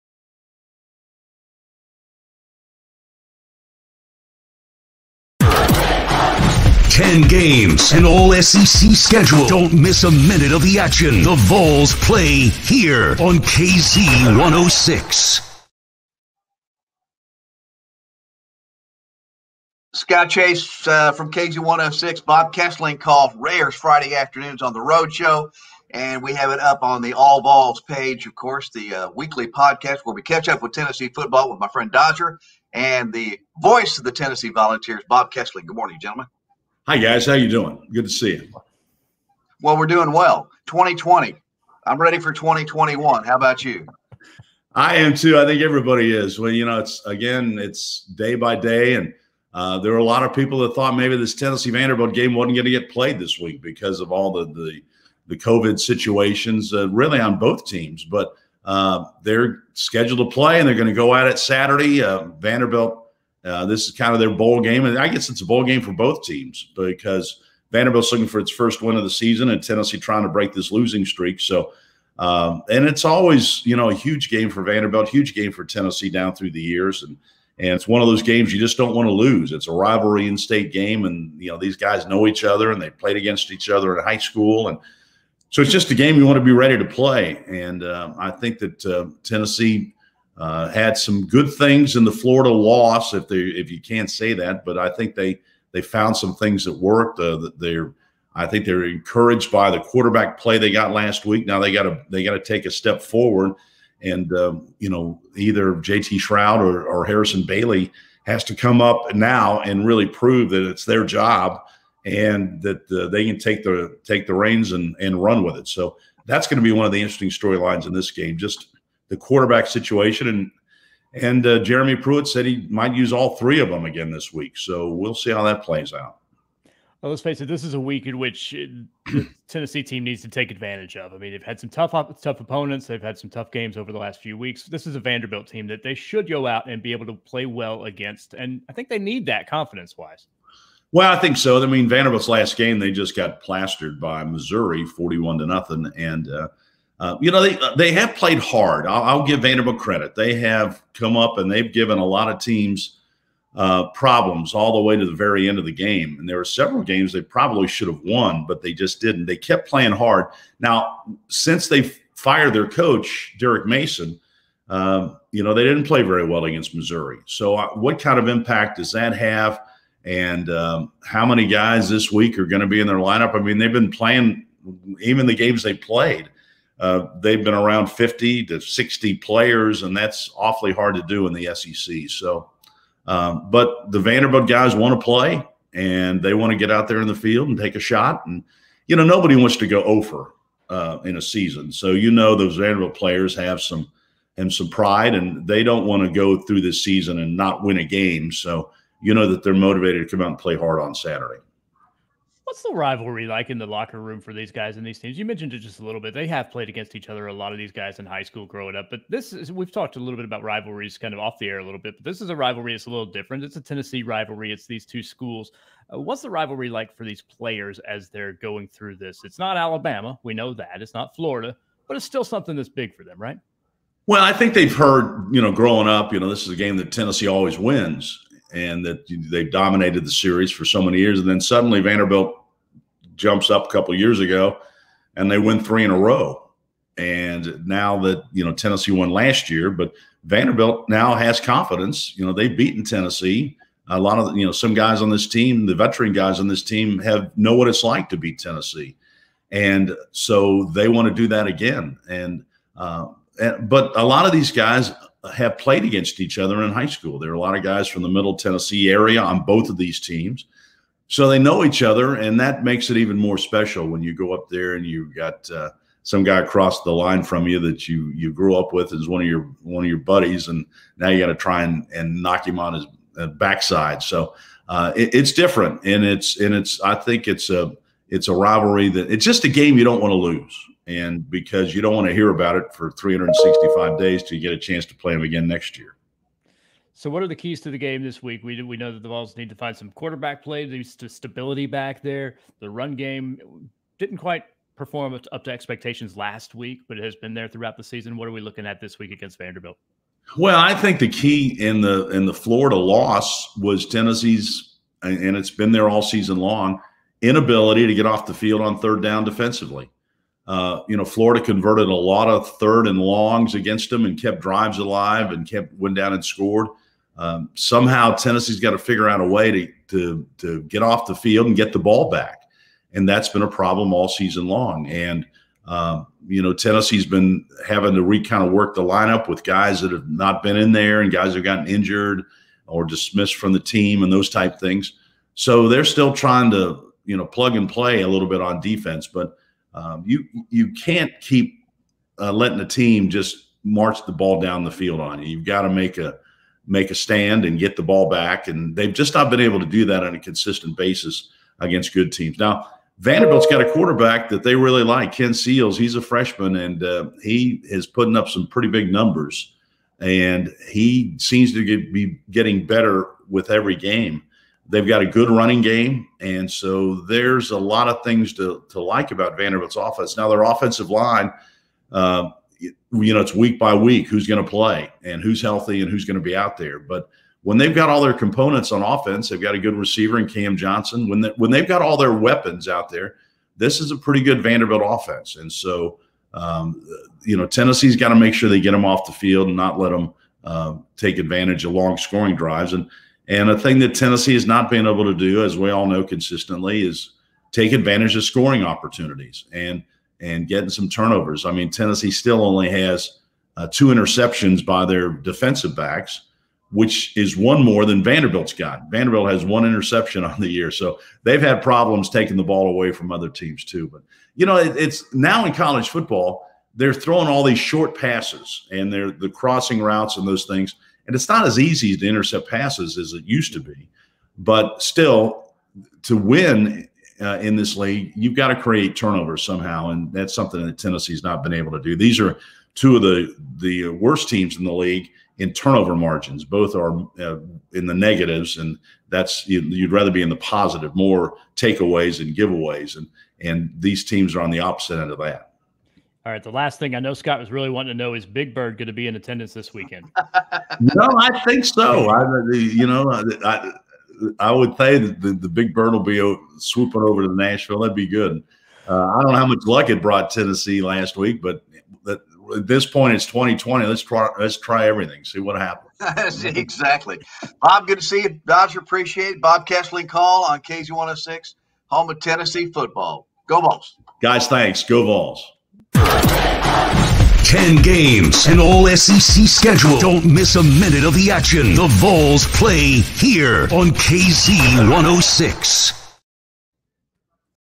Kesling Call: Vanderbilt
Dodger and Scott chat with Bob Kesling about the Vols and Dores.